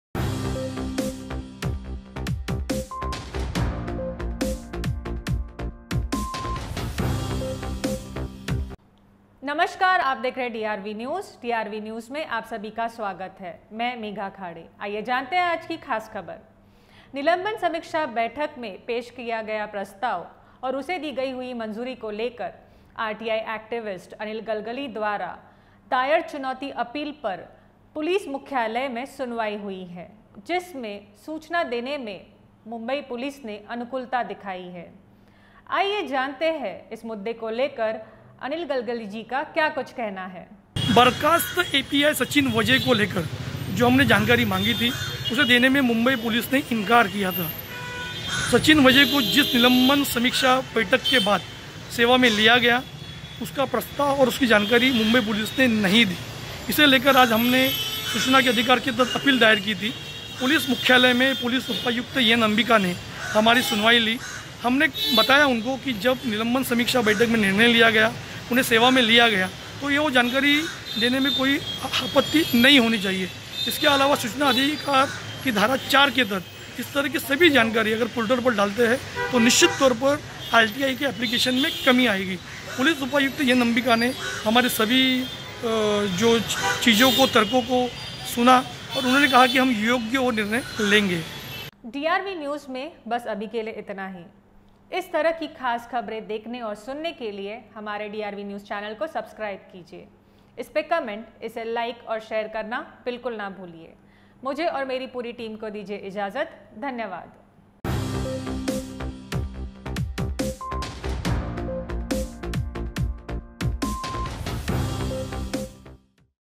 सचिन वाझे की जानकारी देने में अब मुंबई पुलिस अनुकूल || DRVNEWS
सचिन वाझे की जानकारी देने में अब मुंबई पुलिस अनुकूल

पोर्टल पर अपलोड करने में सकारात्मक प्रतिसाद

अनिल गलगली ने वाझे पर जानकारी के लिए..

..8 अप्रैल दायर किया था ऑनलाइन आवेदन
Mumbai Police is now friendly in giving information about Sachin Vajhe

Positive response in uploading on the portal

Anil Galgali for information on Waze..

..8 April was filed online application